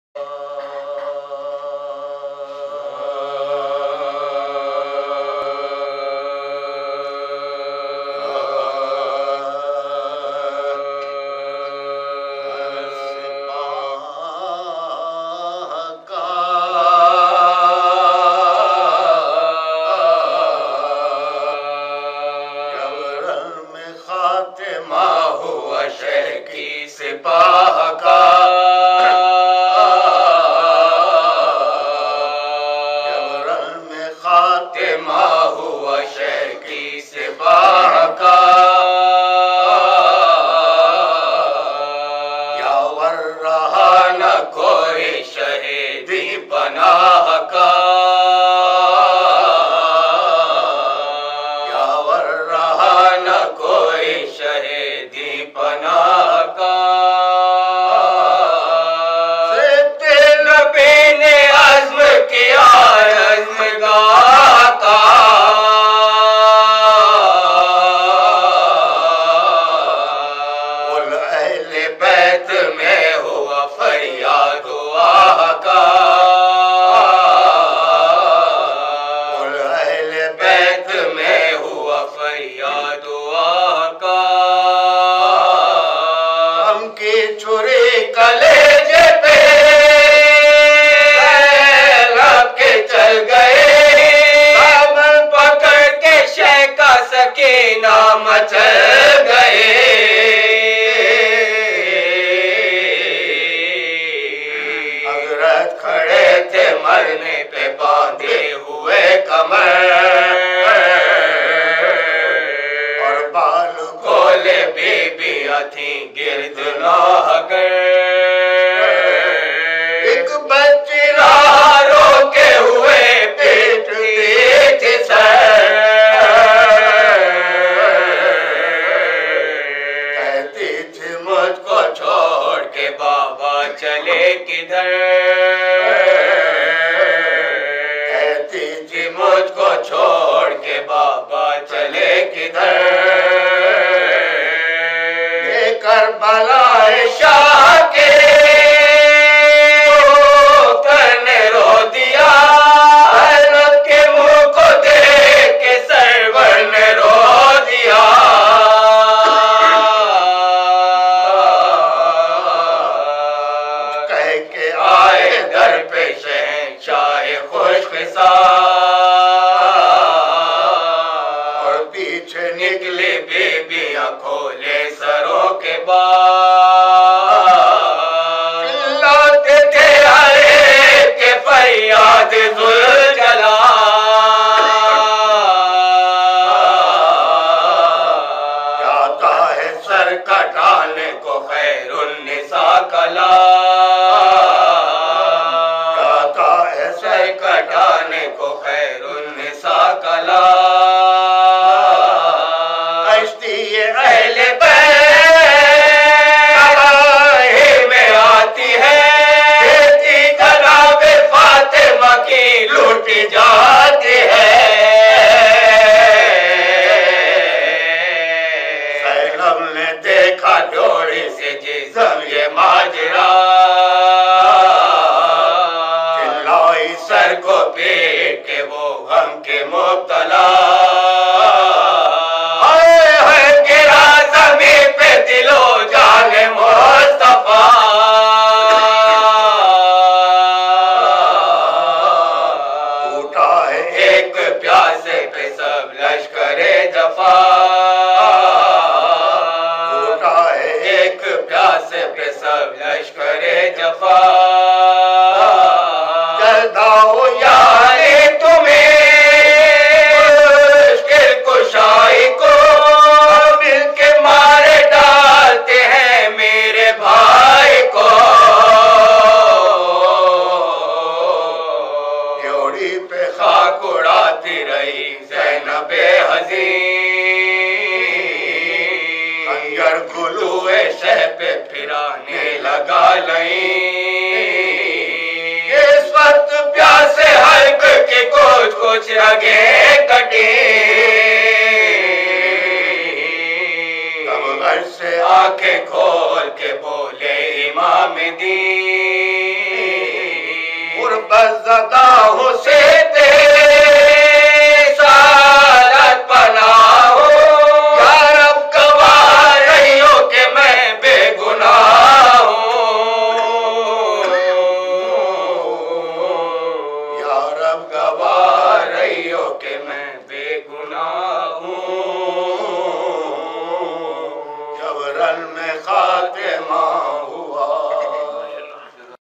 Marsia Hassan Jaffar Zaidi 230116 Majlis Barsi Syed Ibne Abbas Res of Gohar Abbas Abidi St Town
مجلس  ترحیم تیسری برسی سیدابنِ عباس  ابنِ سید ابو طالب
خطابت :  مولانا  ریاض حسین صفوی
12ربیع الثانی  1437 بمطابق  23  جنوری  2016
سوگواران :  خانوادہ  سید  ابنِ عباس
بمقام :  رہایش گاہ پسران مرحوم ابن عباس سیٹلا ئٹ ٹاؤن راولپنڈی
ویڈیو پیشکش :  سید شمشاد حیدر  ایس  ایچ  پروڈکشنز اسلام آباد